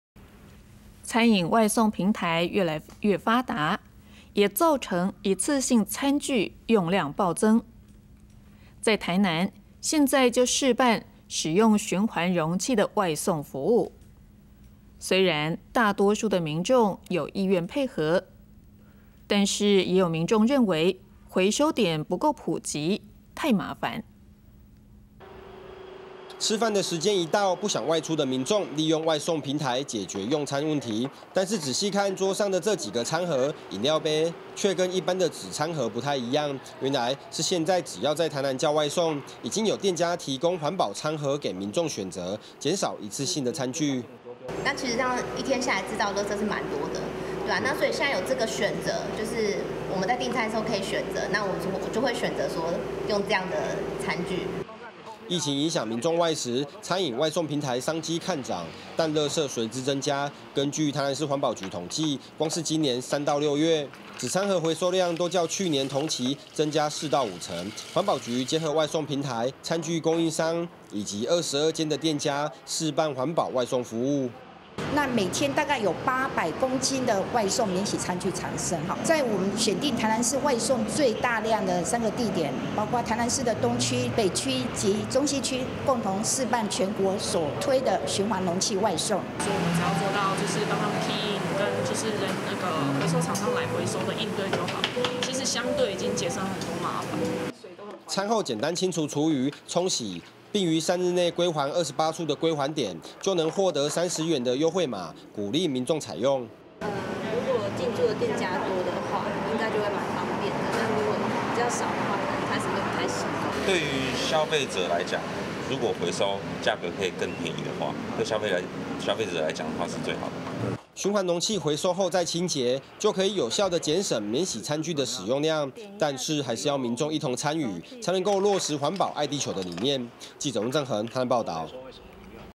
外送平台因疫生意好 一次性餐具用量暴增
20201112 公視手語新聞
公視手語新聞臉書